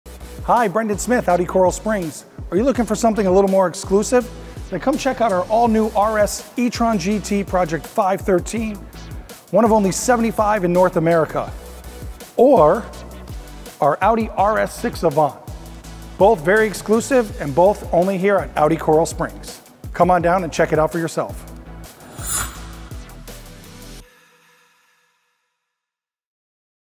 Exclusive Cars Available at Audi Coral Springs!
There are exclusive cars available at Audi Coral Springs!

Learn about them with Brendan Smith! ✅